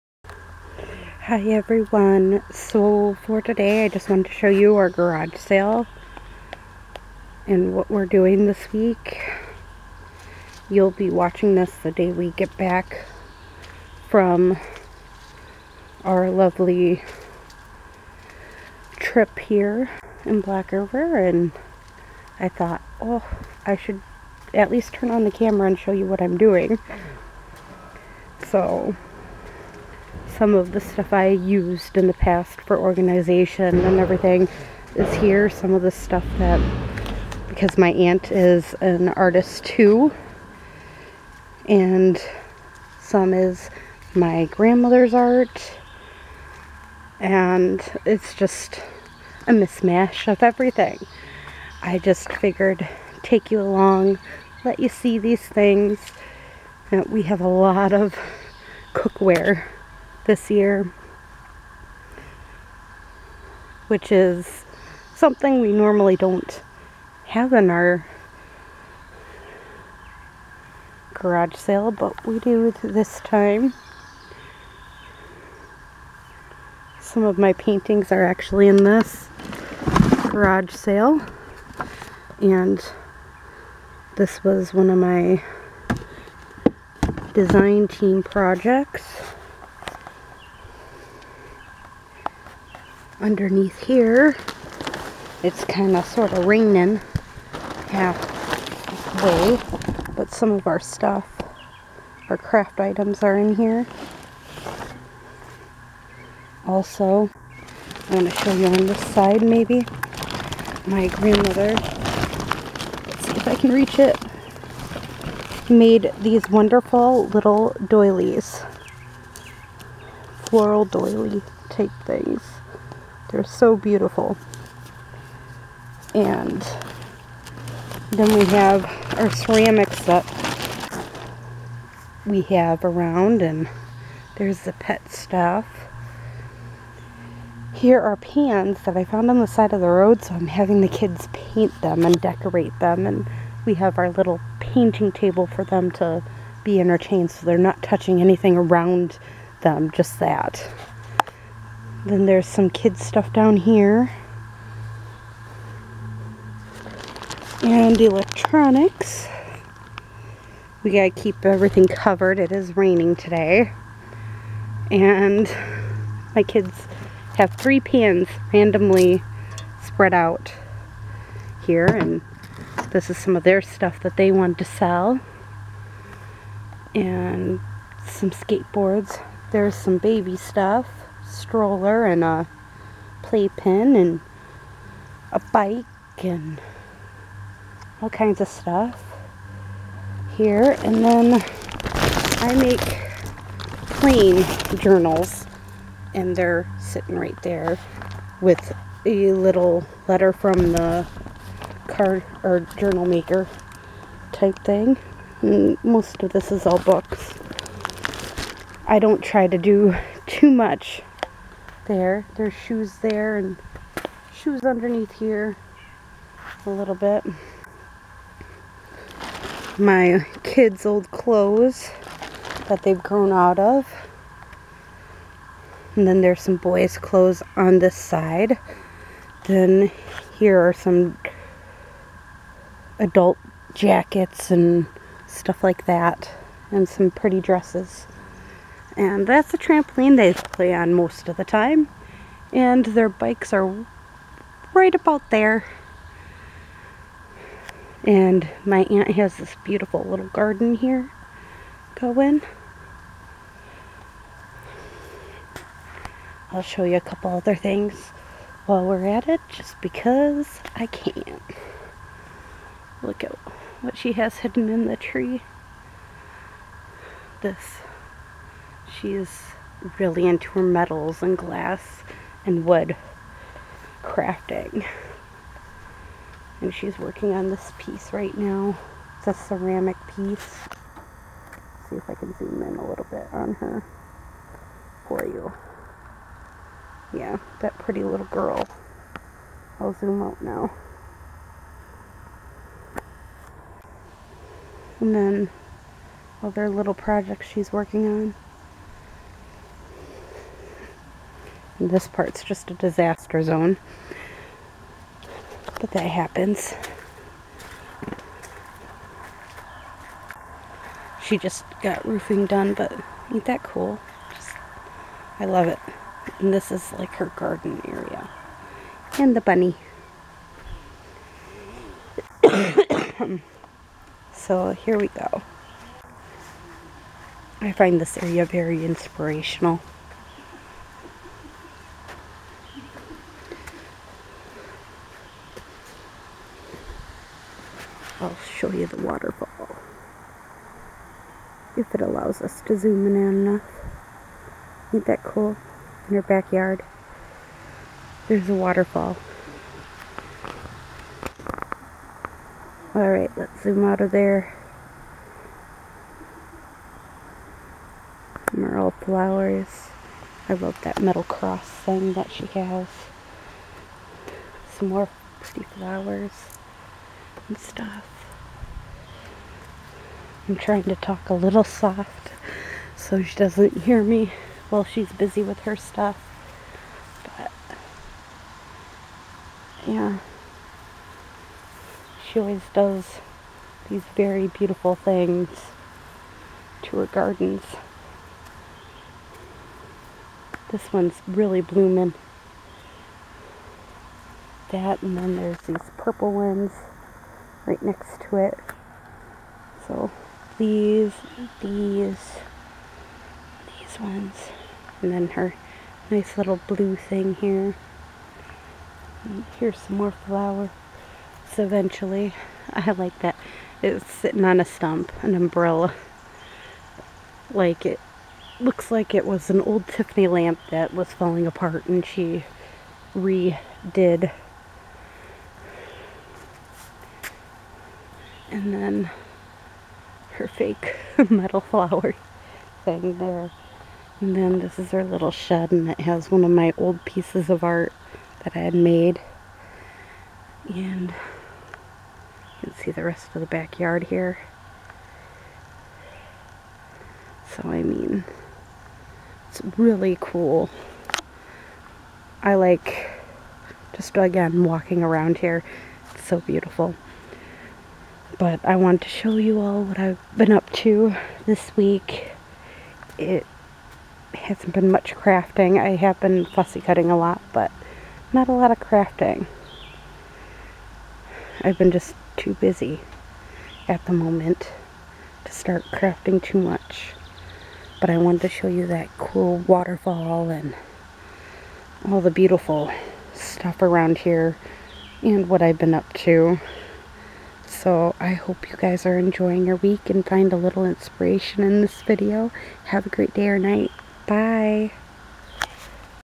Update: Garage Sale/ Garden Walkthrough
I wanted to show what I have been up to over the past week. I wanted to show where I get quite a few of my inspirations. I hope the garden will be inspirational for you too.